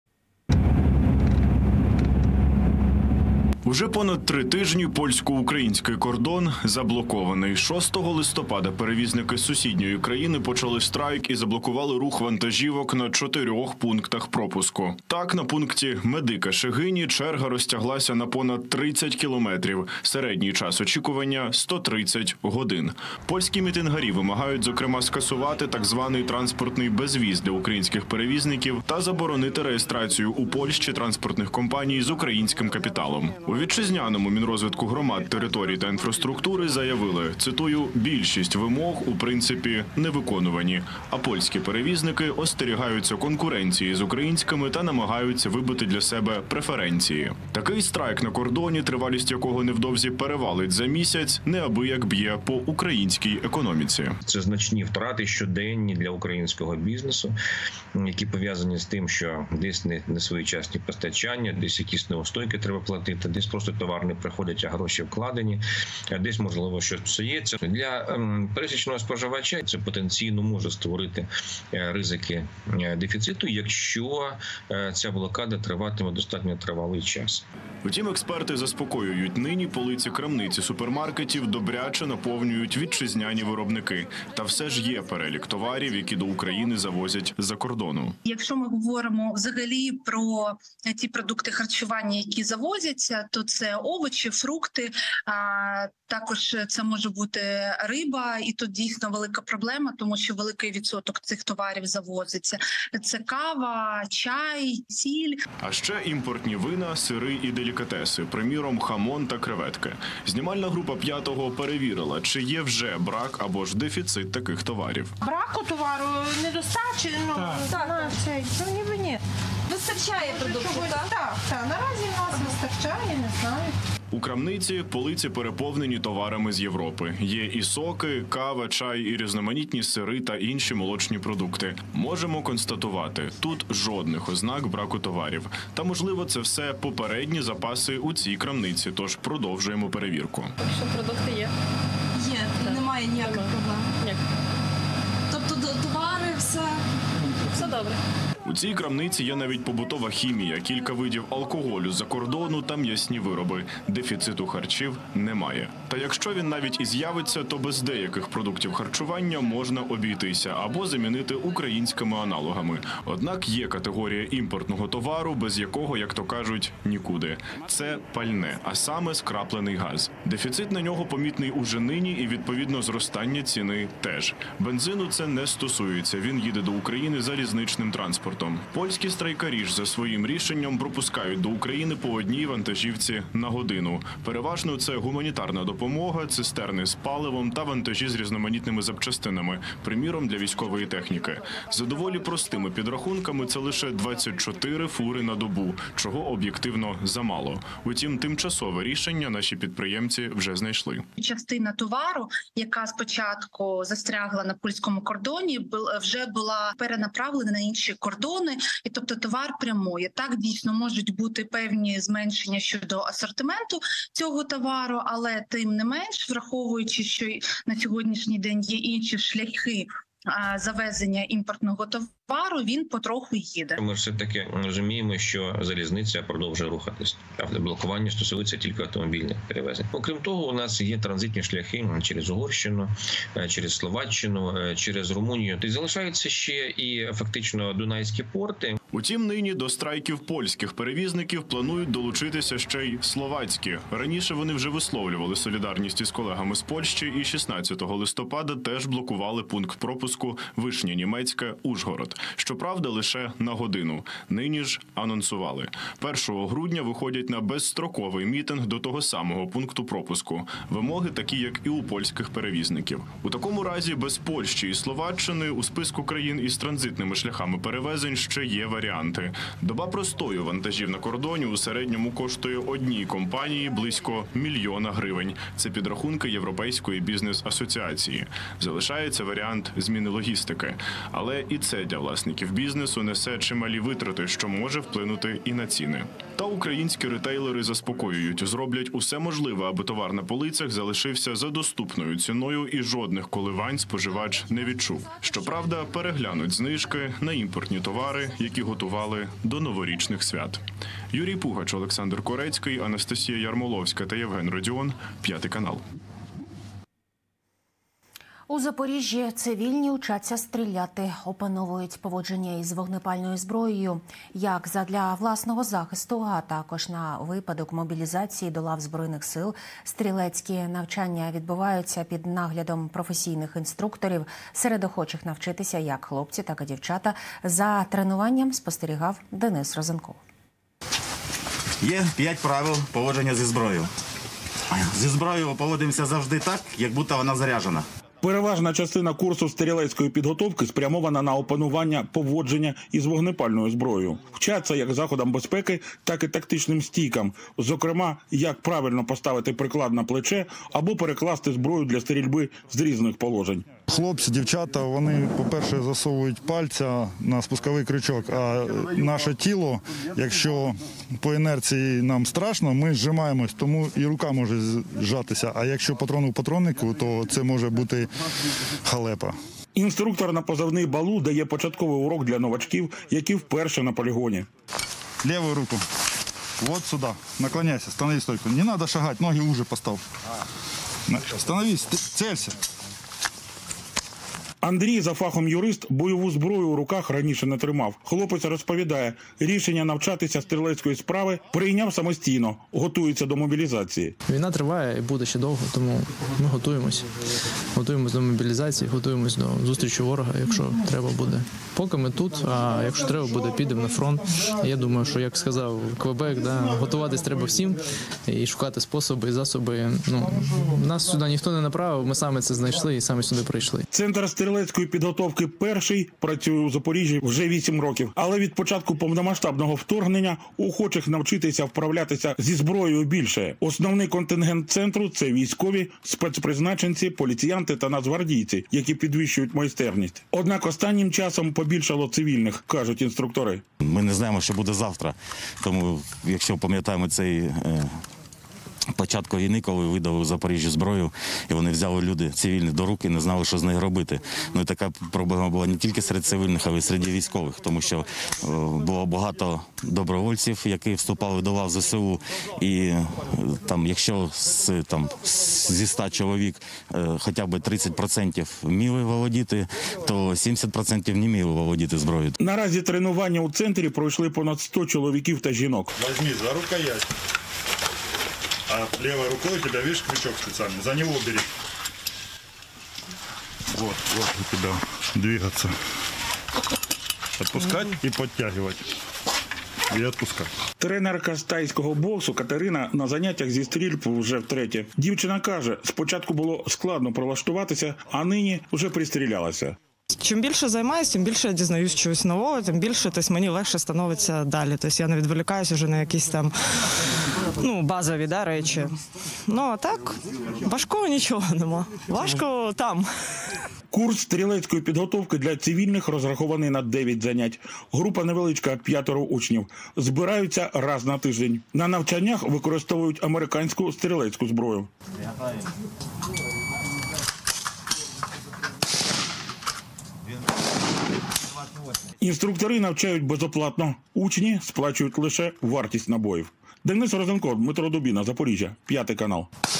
РИЗИК ДЕФІЦИТУ продуктів через страйк польських перевізників? Як вплине на продуктовий кошик?
На Новий рік - без алкоголю, сиру і морепродуктів. Низку товарів не можуть доставити в Україну через страйк і блокування кордону польськими перевізниками. Він триває від початку листопада. Фури з вантажами вишиковуються у черги довжиною у кількадесят кілометрів. Подекуди водіям доводиться чекати до 10-ти днів у складних умовах. Економіка нашої країни вже зазнала збитків на понад 400 мільйонів євро. Такі підрахунки Федерації роботодавців України. Як страйк польських перевізників вплине на продуктовий кошик українців, чи є вже брак або дефіцит імпортних товарів та які  шляхи вирішення цієї проблеми - Юрій Пугач дізнавався.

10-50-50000 - будь-яка сума має значення.

Разом переможемо!
Слава Україні!